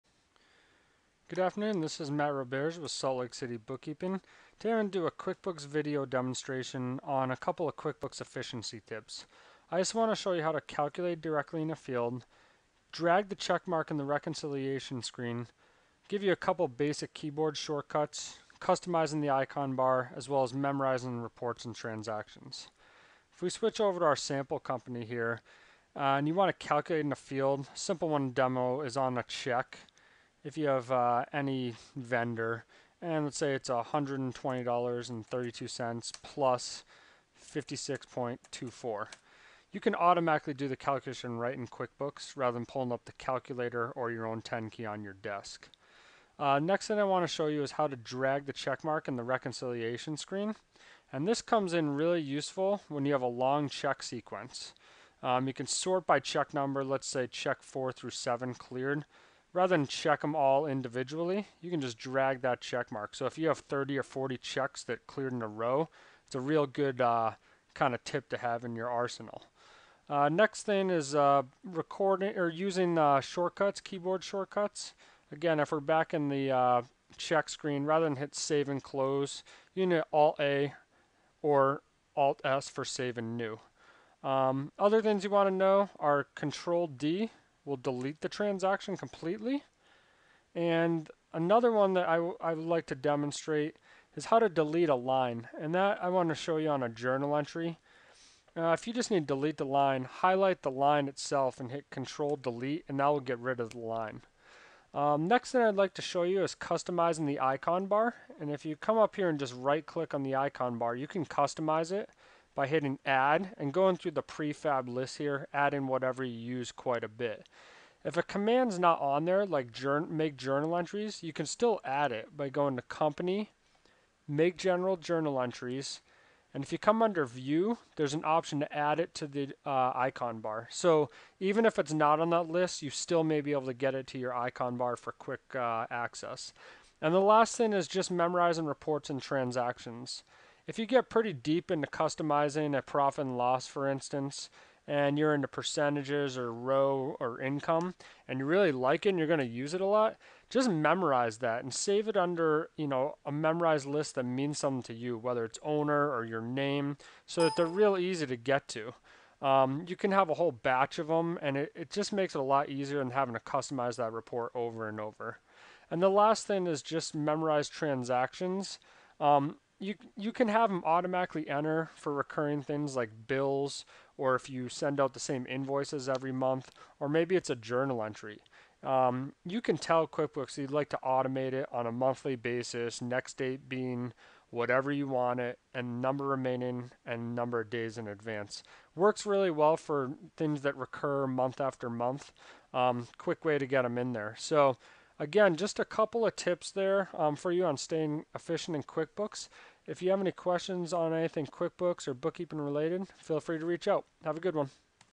QuickBooks Video Tip: QuickBooks Efficiency Tips
with some QuickBooks efficiency tips. There are certain shortcuts and tricks that you can use in QuickBooks that may not seem like a big deal but can save a lot of time. In this video I demonstrate a few of the tactics that I use to stay super efficient in QuickBooks. I will cover such tips as calculating directly in a QuickBooks field, dragging the cleared status in the reconciliation screen, QuickBooks keyboard shortcuts, memorized reports and transactions, and customizing the QuickBooks icon bar.